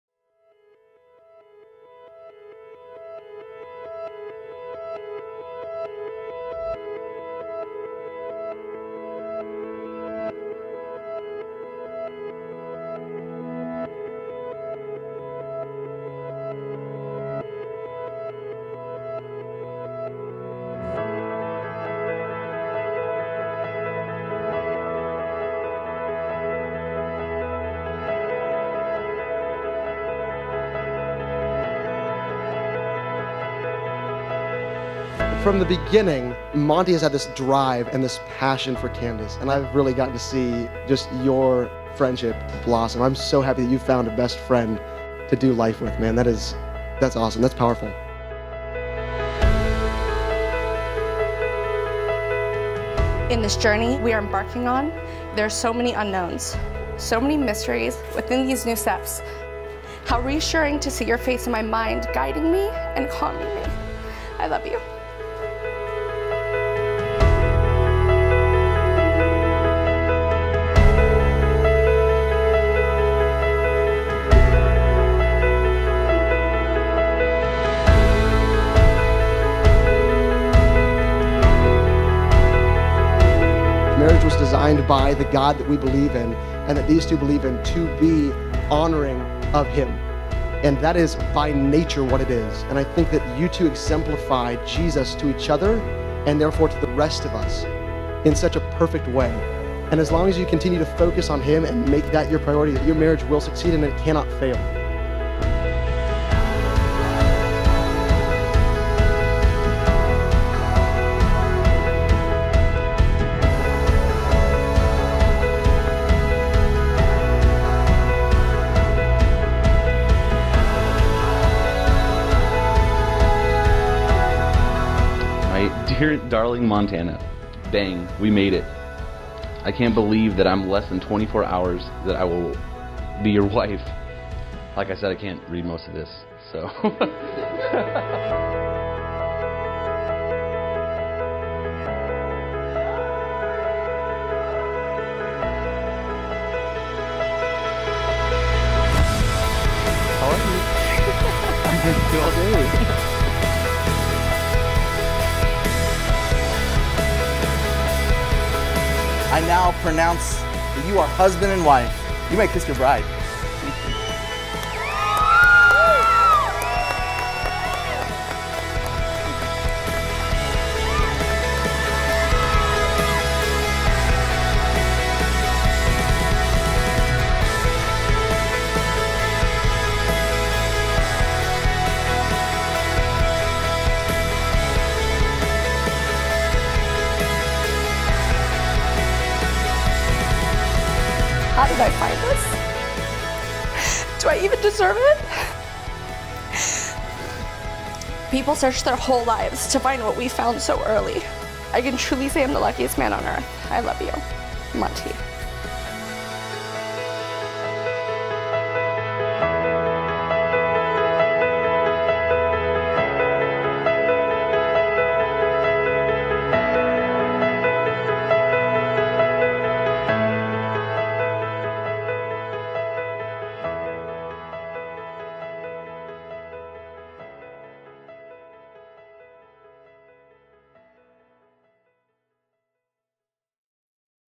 Grooms Letter to Bride is adorable! Must watch this Wedding Video!
Groom gives the most adorable letter to his bride! Must watch this wedding video :)